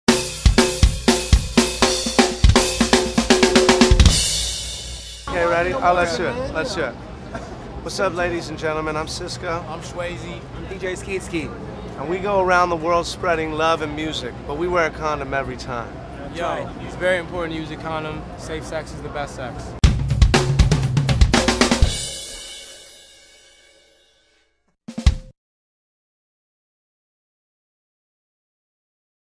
Trojan_VansWarpedTour_shwayze.mov
Testimonial from various band during the Vans Warped Tour about the sexual health crisis in America